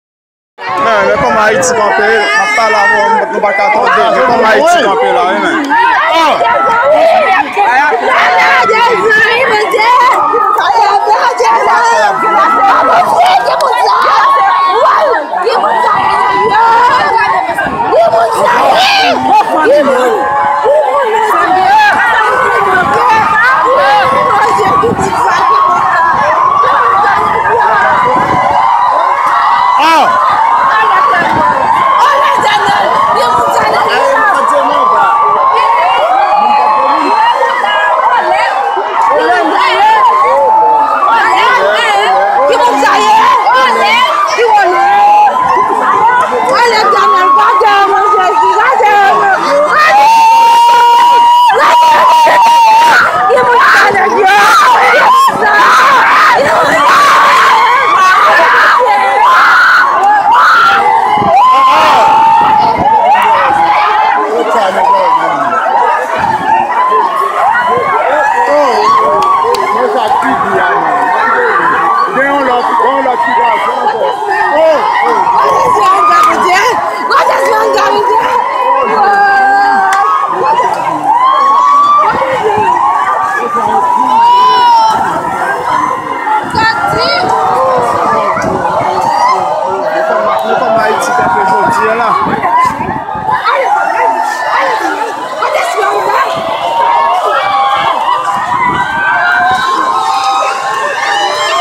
Plis pase 20 moun mouri pami yo 2 tibebe
Nofraj Yon tibatiman nan zile LATORTUE